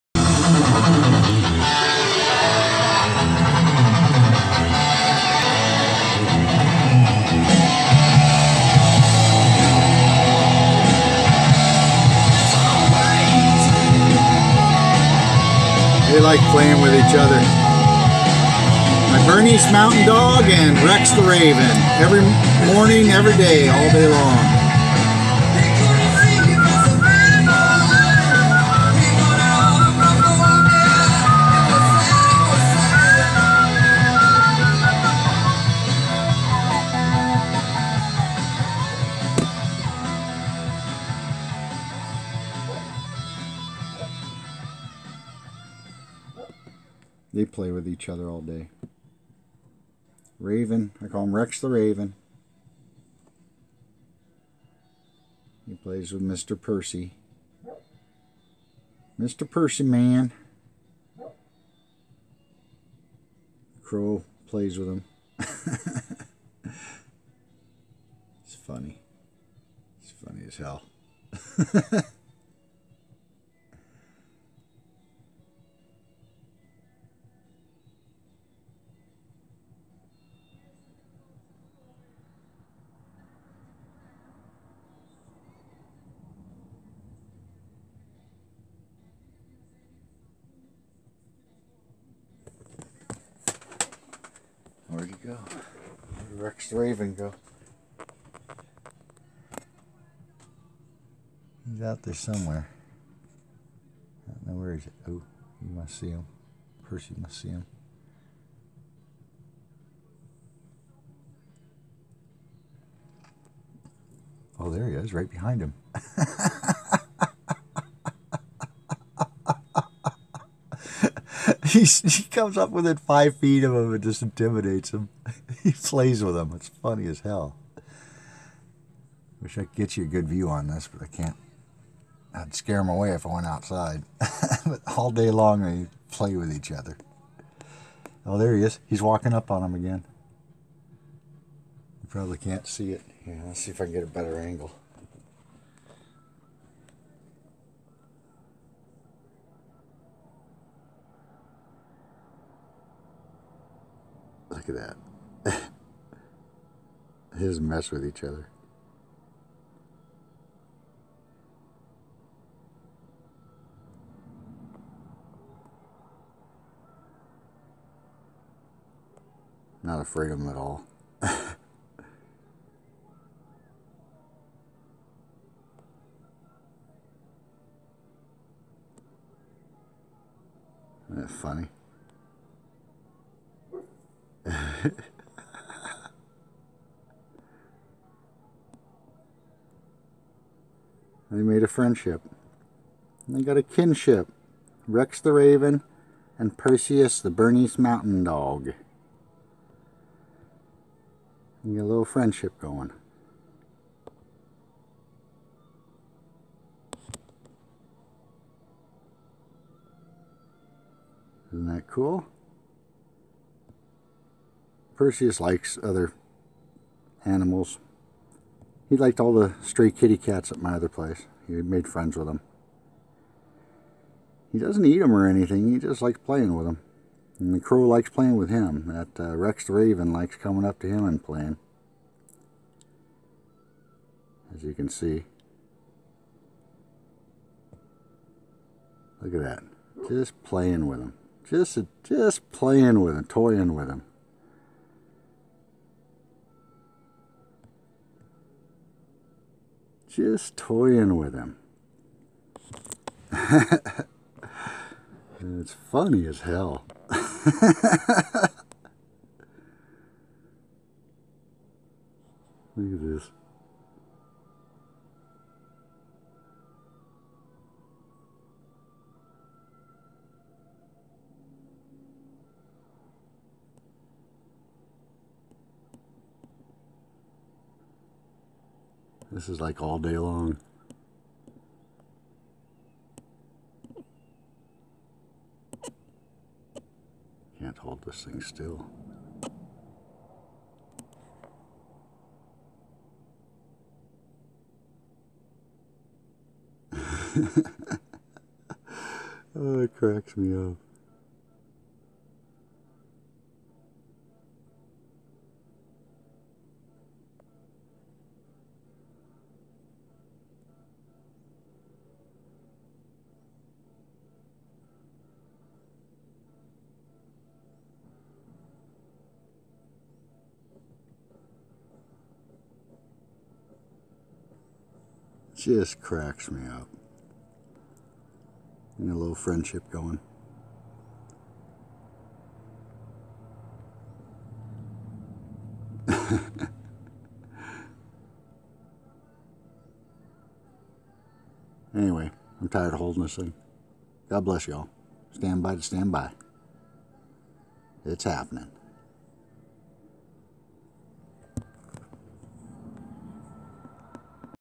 Rex the Raven vs Perseus the Bernese Mountain Dog.
Just another typical day in the end of times. Friendship between Rex The Raven and Perseus the Bernese Mountain Dog